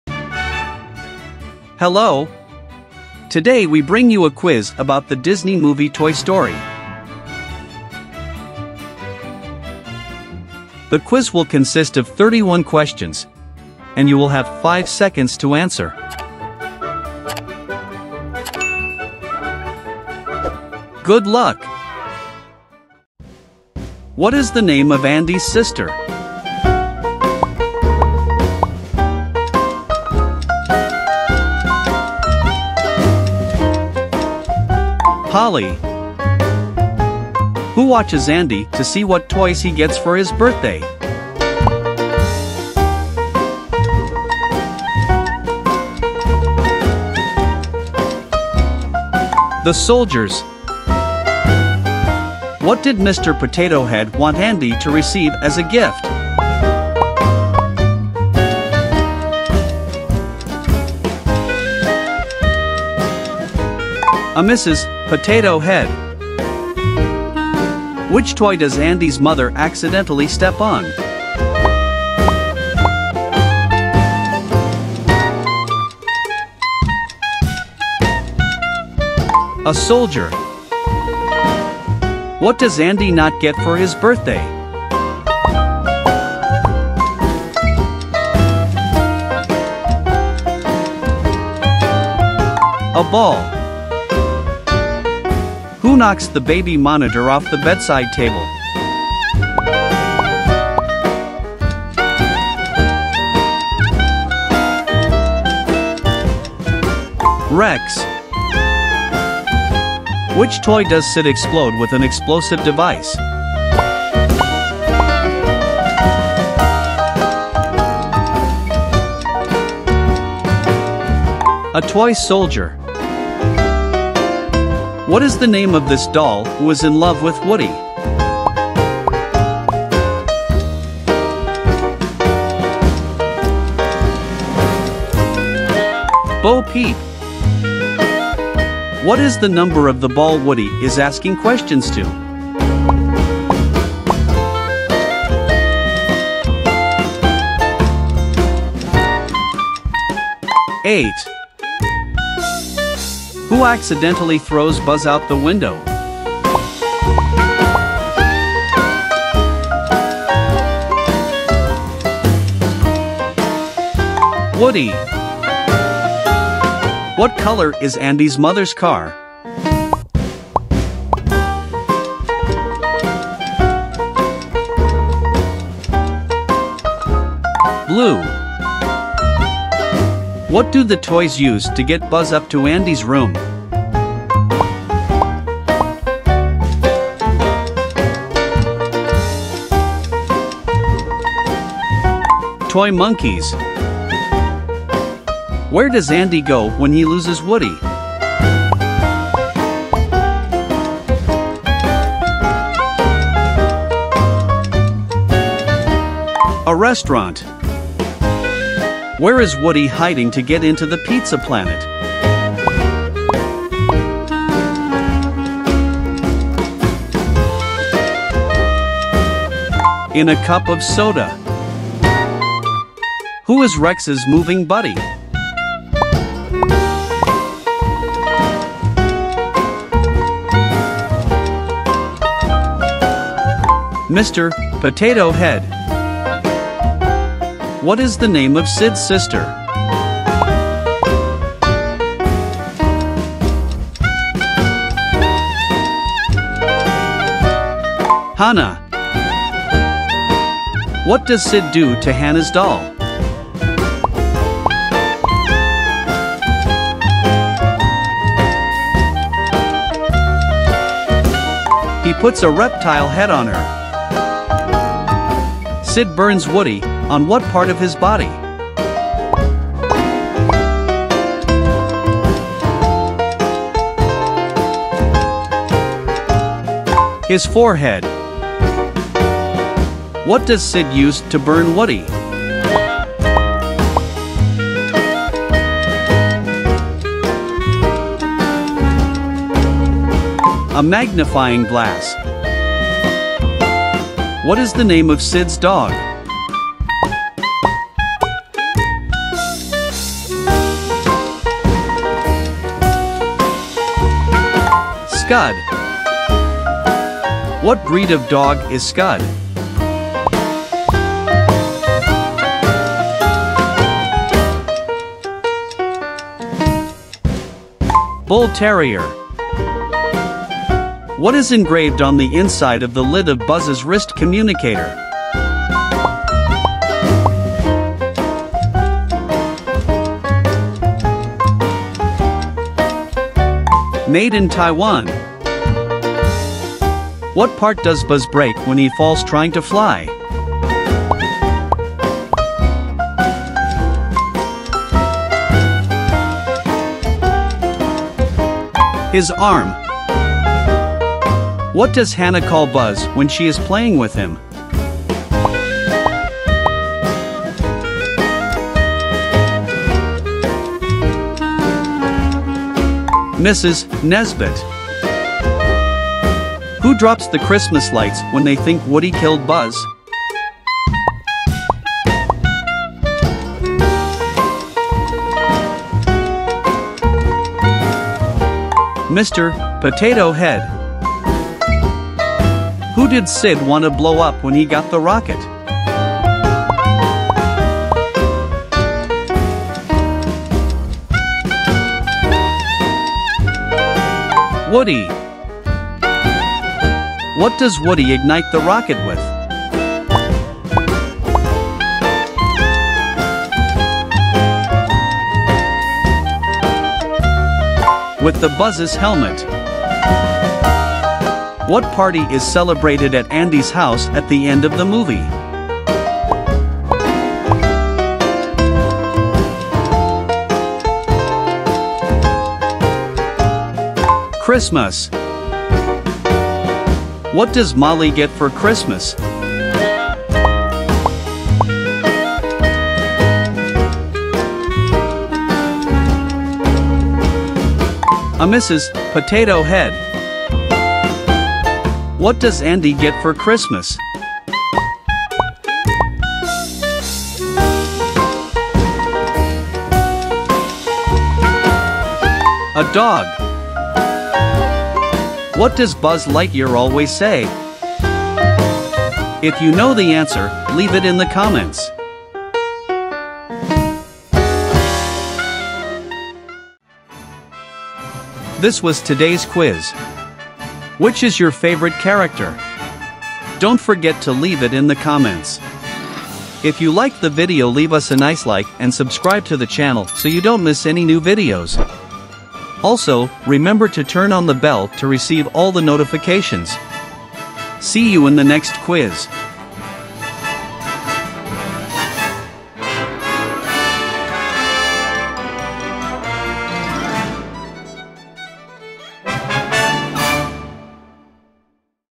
"TOY STORY" QUIZ! | How Much Do You Know About "TOY STORY"? | MOVIE QUIZ/TRIVIA/CHALLENGE
This is a Quiz about the 1995 Disney movie "TOY STORY". If you like this Pixar movie, don't miss this trivia quiz and find out how well you know it!

👇IF YOU LIKED THE VIDEO, DON'T FORGET 👇
🔵GIVE YOUR LIKE! 👍